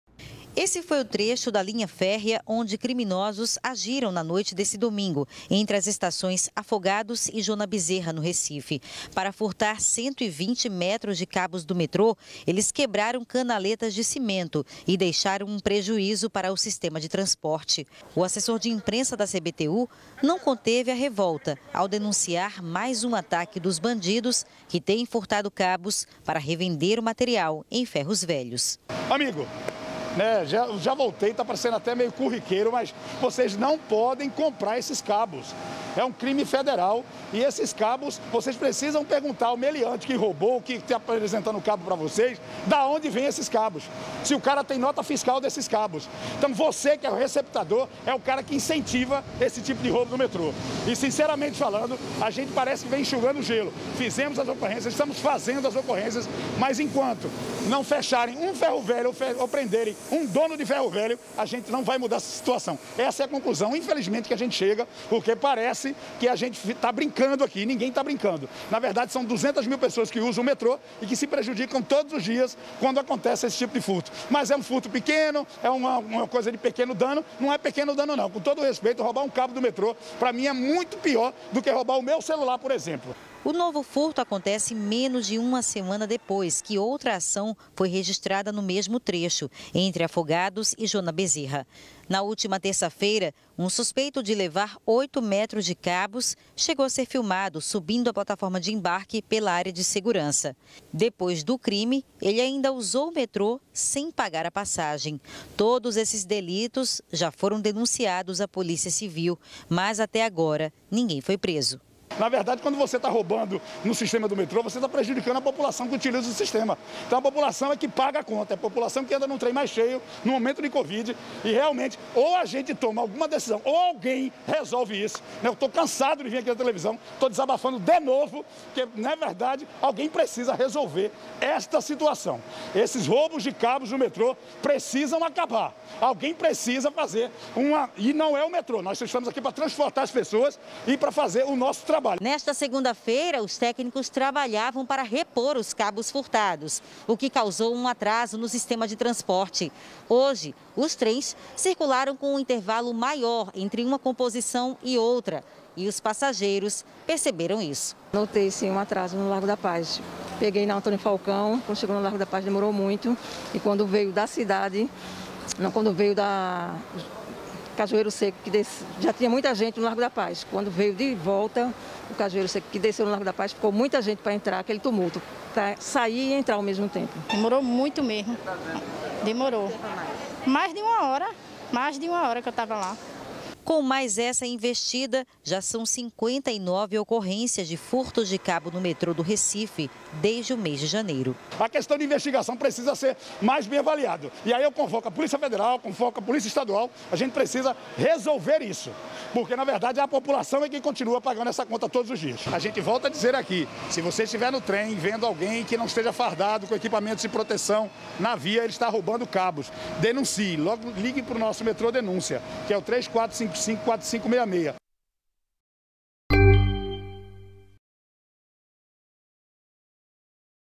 FIOS DO METRÔ SÃO ROUBADOS NOVAMENTE
Bandidos roubam cabos do metrô do Recife e deixam trens da Linha Sul e Centro mais lentos.